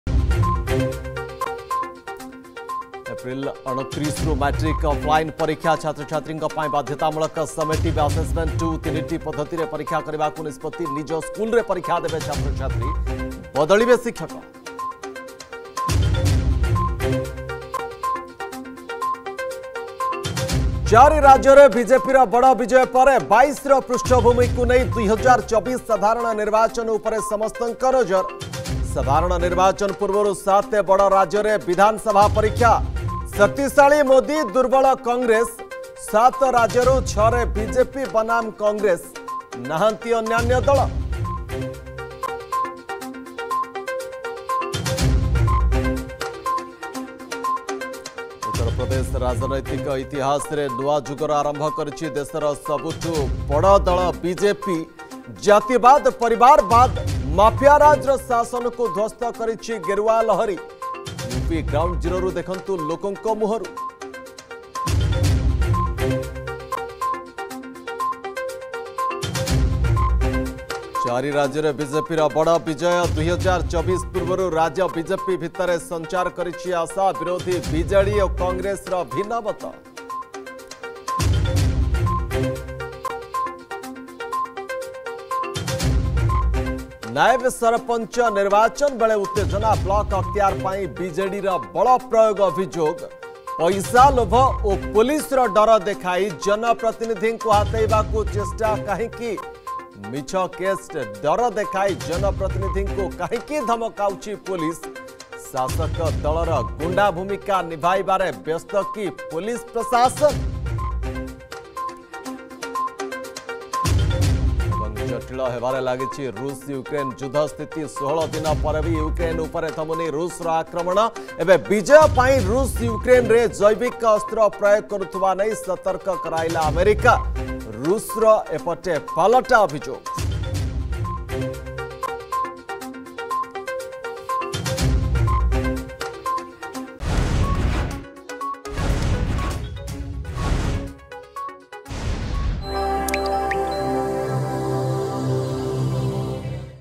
Headlines 7 pm । 11 mar 2022 । ArgusNews
+ଏପ୍ରିଲ ୨୯ରୁ ମାଟ୍ରିକ ଅଫଲାଇନ୍ ପରୀକ୍ଷା
+ଏବଂ ଜଟିଳ ହେବାରେ ଲାଗିଛି ରୁଷ-ୟୁକ୍ରେନ ଯୁଦ୍ଧ ସ୍ଥିତି
+ନାଏବ ସରପଞ୍ଚ ନିର୍ବାଚନ ବେଳେ ଉତ୍ତେଜନା
+୪ ରାଜ୍ୟରେ ବିଜେପିର ବଡ ବିଜୟ